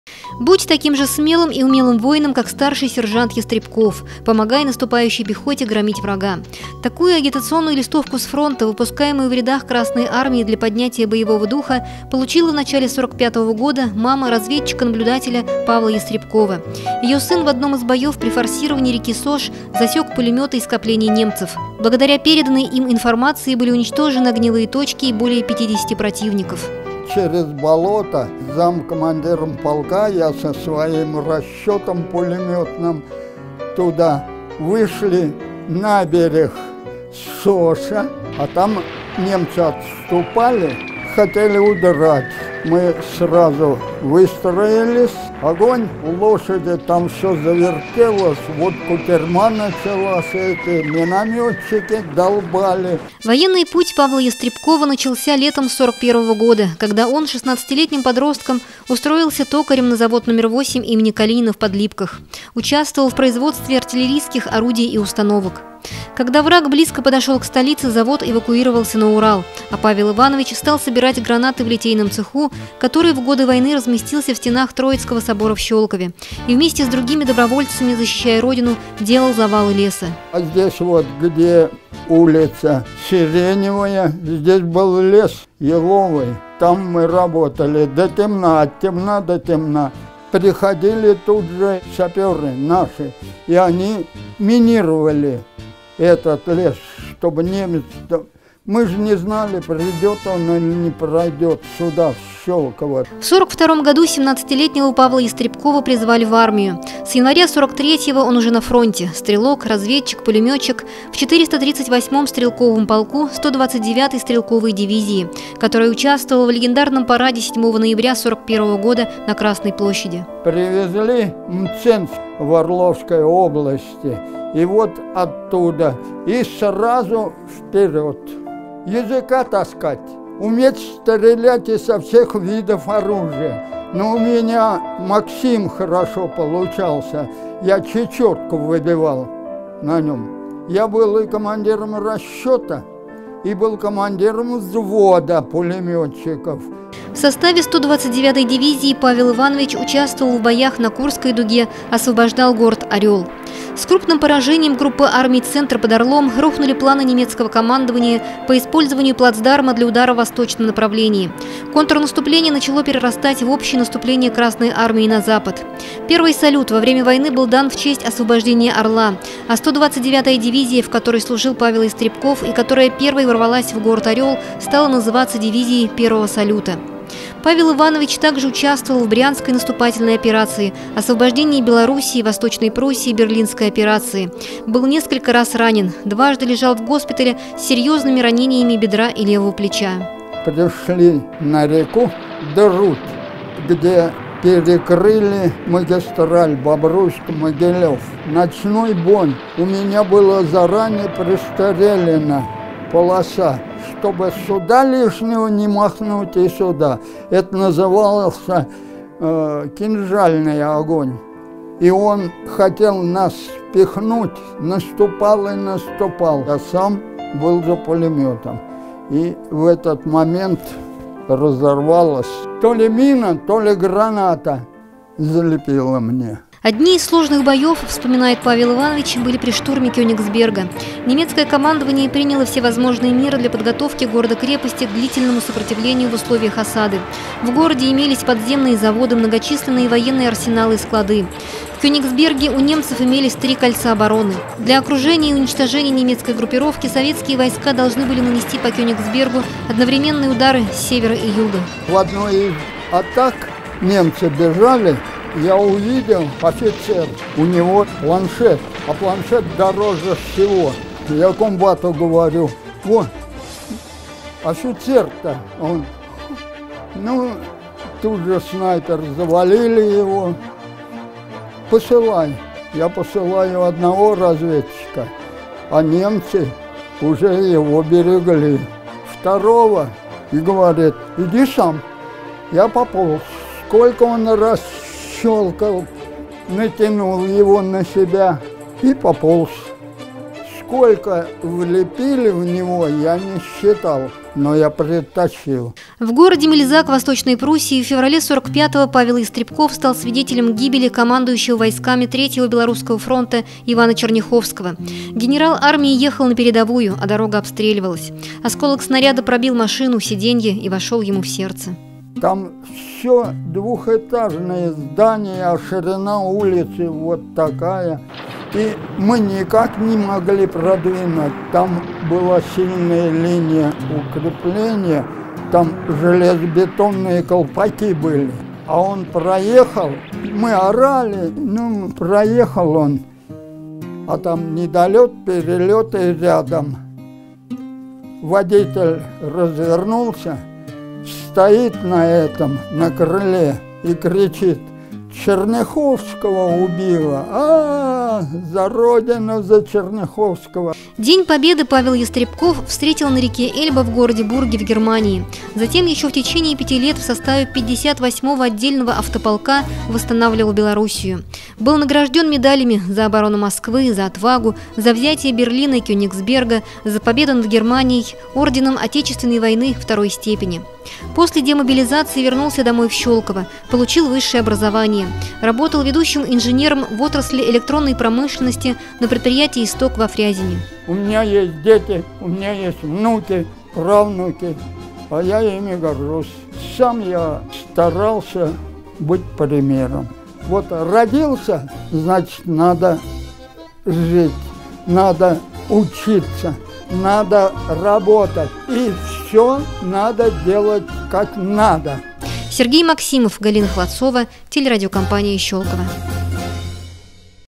Ветеран Павел Ястребков
5 декабря в нашей стране будет отмечаться крупная дата – 75-летие битвы под Москвой. Этот день стал переломным в ходе Великой Отечественной войны и истории. Советские войска перешли в контрнаступление и положили  начало разгрому фашистских войск. Герой нашего сюжета, участник Великой Отечественной войны Павел Ястребков, в дни битвы под Москвой участвовал в производстве артиллерийских орудий и установок, собирал гранаты. С января 43-го года и до самой Победы защищал Родину на передовой фронта.